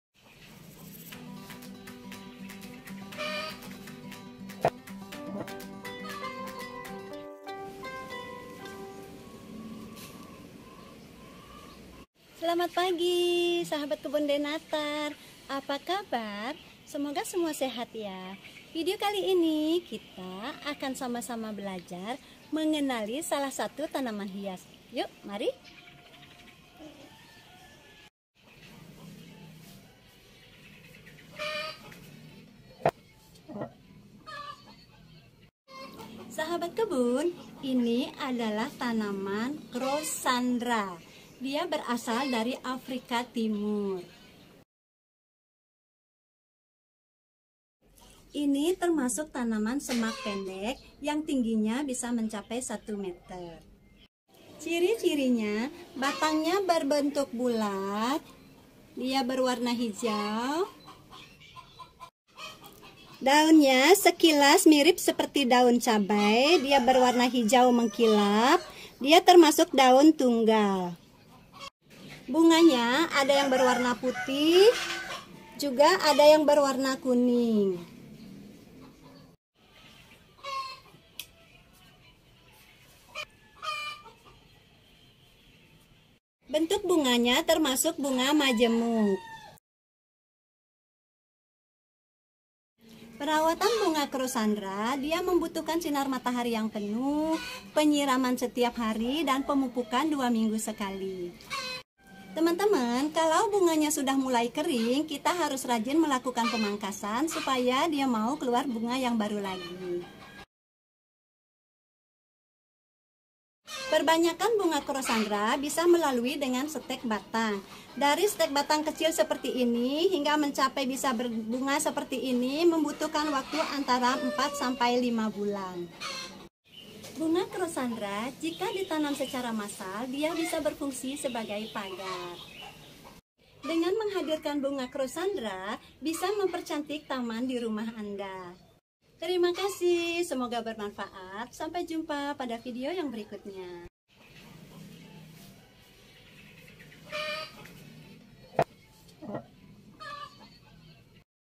Bunga Krosandra Tanaman Hias Pagar Pembatas (Crossandra infundibuliformis)
Pada video kali ini Saya akan menjelaskan salah satu tanaman hias bunga yang berasal dari Afrika Timur. Yap Crossandra infundibuliformis, dikenal dengan nama bunga krosandra adalah tanaman hias bunga yang kan kita bahas hari ini. Yuk disimak videonya.

Apabila Sahabat mempunyai saran dan masukan silahkan tulis dikolom komentar yaa.

Salam Hangat
Kebun de Natar